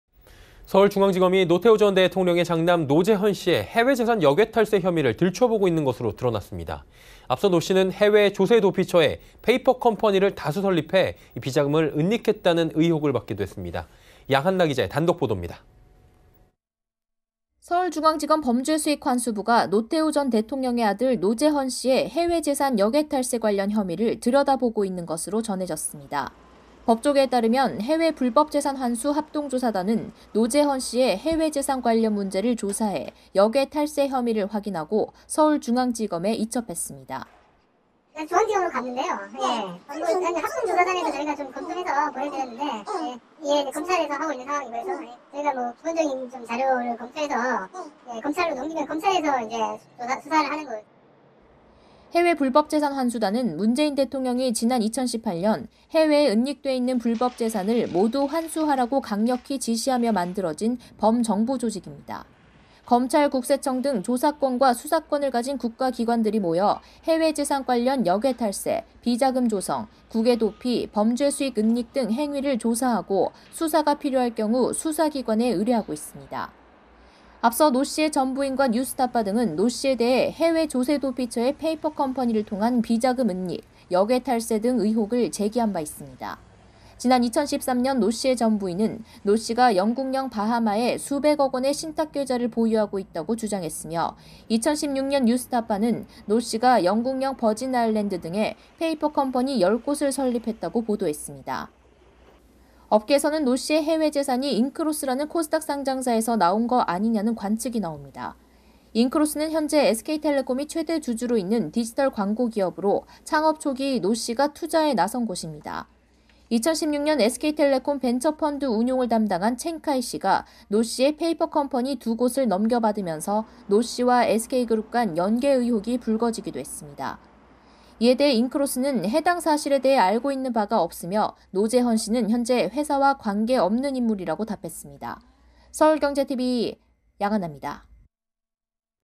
중앙지검, 노태우 장남 역외탈세 혐의 들춘다
[앵커]
서울중앙지검이 노태우 전 대통령의 장남 노재헌씨의 해외재산 역외탈세 혐의를 들춰보고 있는 것으로 드러났습니다. 앞서 노씨는 해외 조세도피처에 페이퍼컴퍼니를 다수 설립해 비자금을 은닉했다는 의혹을 받기도 했습니다. 양한나기자의 단독보도입니다.

[기자]
서울중앙지검 범죄수익환수부가 노태우 전 대통령의 아들 노재헌씨의 해외재산 역외탈세 관련 혐의를 들여다보고 있는 것으로 전해졌습니다.

법조계에 따르면 해외불법재산환수 합동조사단은 노재헌씨의 해외재산 관련 문제를 조사해 역외탈세 혐의를 확인하고 서울중앙지검에 이첩했습니다.

[인터뷰] 해외불법재산환수 합동조사단 관계자
“중앙지검으로 갔는데요… 합동조사단에서 저희가 검토해서 보내드렸는데 (현재) 검찰에서 하고 있는 상황이고요…저희가 기본적인 자료를 검토해서 검찰로 넘기면 검찰에서 수사를 하는…”

해외불법재산환수단은 문재인 대통령이 지난 2018년 ‘해외에 은닉돼 있는 불법 재산을 모두 환수하라’고 강력히 지시하며 만들어진 범정부조직입니다.

검찰, 국세청 등 조사권과 수사권을 가진 국가기관들이 모여 해외재산 관련 역외탈세, 비자금 조성, 국외도피, 범죄수익은닉 등 행위를 조사하고 수사가 필요할 경우 수사기관에 의뢰하고 있습니다.

앞서 노씨의 전 부인과 뉴스타파 등은 노씨에 대해 해외 조세도피처의 페이퍼컴퍼니를 통한 비자금 은닉, 역외 탈세 등 의혹을 제기한 바 있습니다.

지난 2013년 노씨의 전 부인은 노씨가 영국령 바하마에 수백억 원의 신탁계좌를 보유하고 있다고 주장했으며 2016년 뉴스타파는 노씨가 영국령 버진아일랜드 등에 페이퍼컴퍼니 10곳을 설립했다고 보도했습니다.

업계에서는 노씨의 해외재산이 인크로스라는 코스닥 상장사에서 나온 것 아니냐는 관측이 나옵니다.

인크로스는 현재 SK텔레콤이 최대주주로 있는 디지털 광고 기업으로 창업 초기 노씨가 투자에 나선 곳입니다.

2016년 SK텔레콤 벤처펀드 운용을 담당한 첸 카이씨가 노씨의 페이퍼컴퍼니 2곳을 넘겨 받으면서 노씨와 SK그룹간 연계 의혹이 불거지기도 했습니다.

이에 대해 인크로스는 “해당 사실에 대해 알고 있는 바가 없으며 노재헌씨는 현재 회사와 관계 없는 인물”이라고 답했습니다. 서울경제TV 양한나입니다.